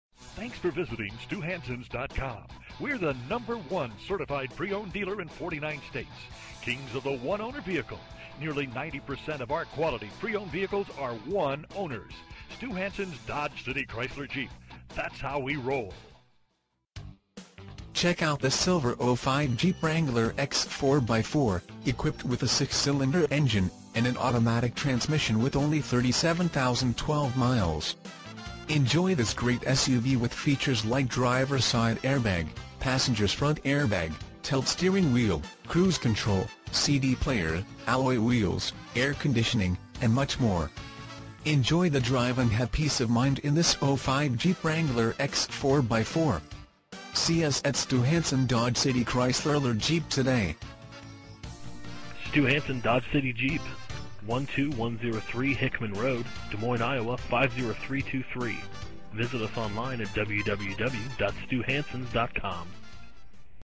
2005 Jeep Wrangler X 4X4 Stew Hansen Dodge City Chrysler
Stew Hansen Dodge City Chrysler Jeep

2005 Jeep Wrangler X    4X4

 37,012 Miles

VIN - 1J4FA39S55P370107

Imagine driving this Silver 2005 Jeep Wrangler X    4X4, equipped with a 6 Cyl. engine  and an automatic transmission with  only 37,012 miles. Enjoy this great SUV with features like Driver Side Air Bag, Passenger's Front Airbag, Tilt Steering Wheel, Cruise Control, CD Player, Alloy Wheels, Air Conditioning, and much more. Enjoy the drive and have peace of mind in this 2005 Jeep Wrangler X    4X...